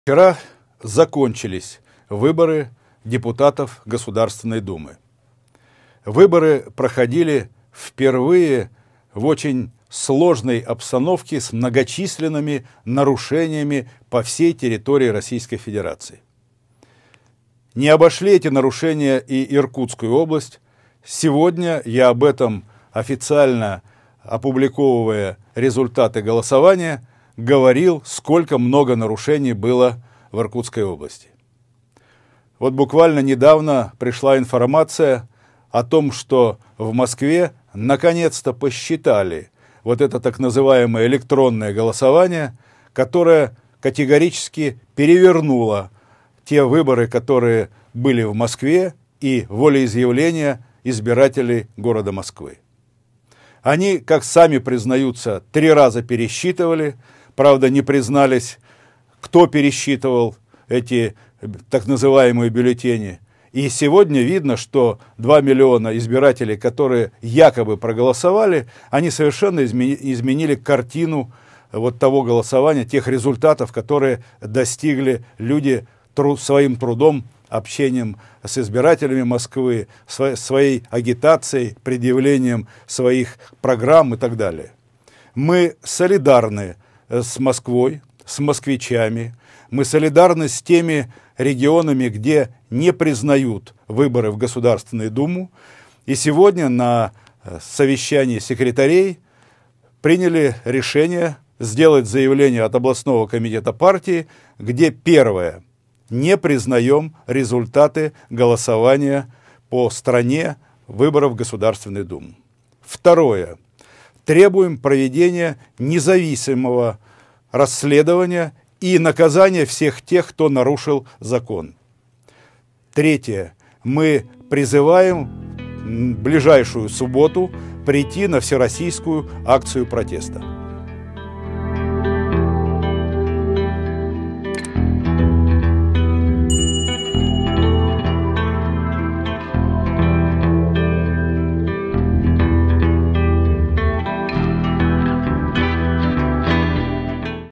КПРФ не признает результаты выборов! Обращение Сергея Левченко
КПРФ не признает результатов выборов, требует провести расследование всех нарушений и объявляет об акции протеста! Обращение Сергея Левченко.

Друзья, буду благодарен Вам за поддержку моей работы - Raiffeisen BANK  - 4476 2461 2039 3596